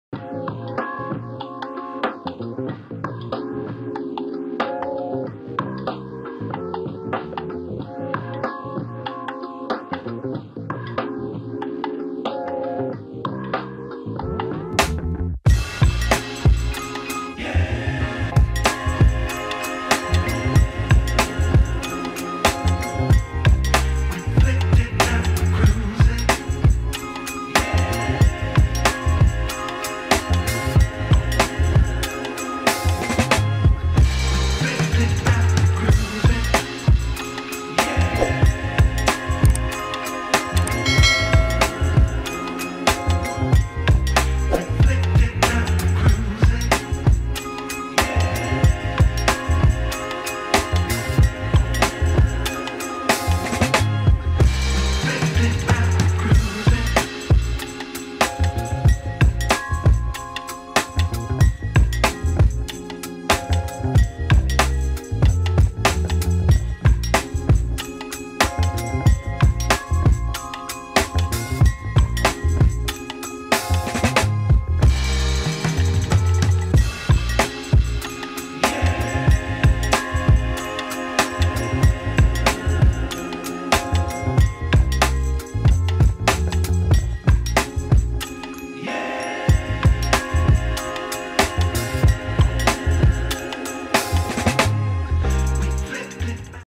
Using a camera from 1913 - Voigtlander Avus 9x12
Let's have fun with this old camera.